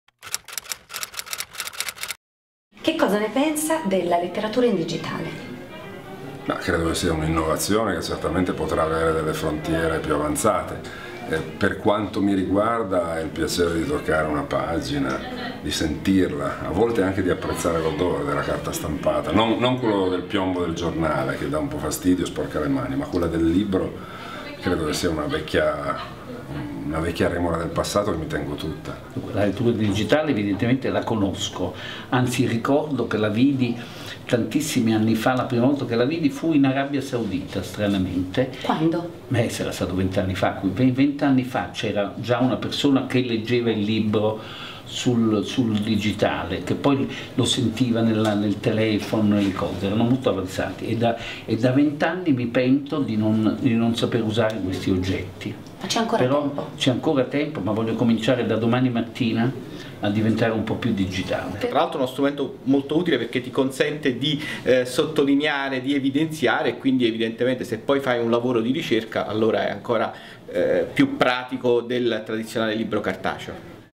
Capalbio Libri 2012 - Gennaro Sangiuliano, "Scacco allo zar"
Intervista a Gennaro Sangiuliano, autore di "Scacco allo zar"" edito da Mondadori, durante la manifestazione letteraria Capalbio Libri 2012.